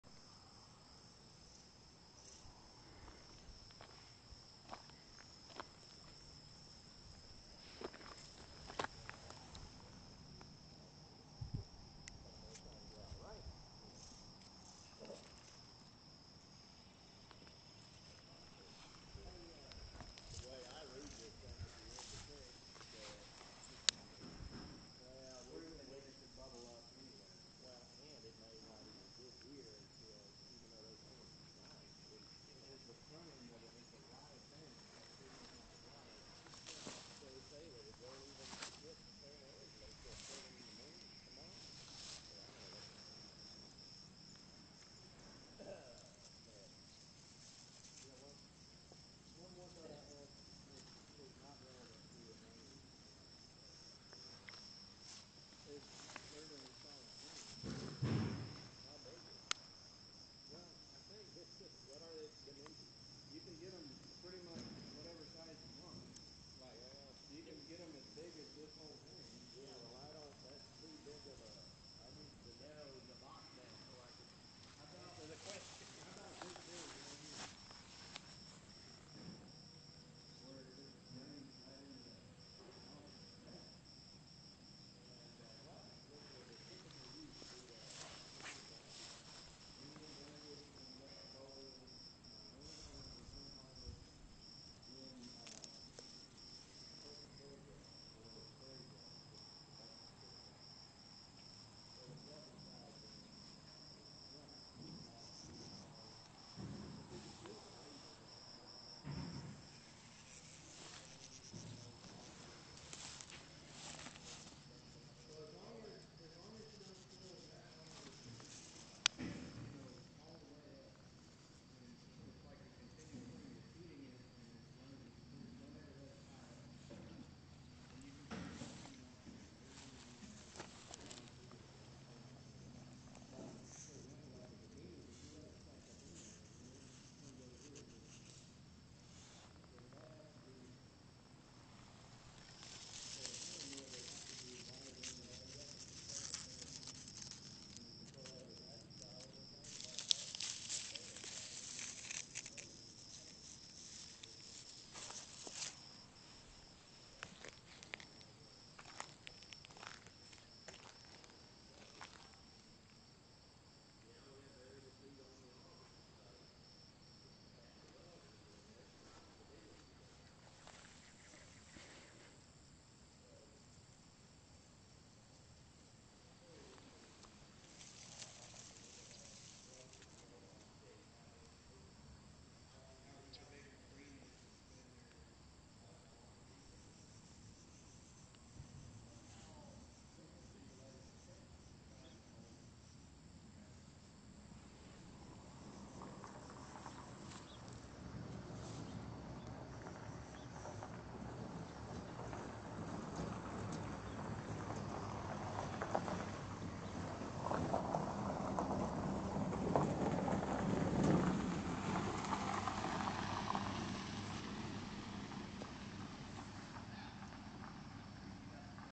Pocket Prairie outside Native American Seed
Little spot outside our office (used to be a parking lot) that has a good amount of native diversity!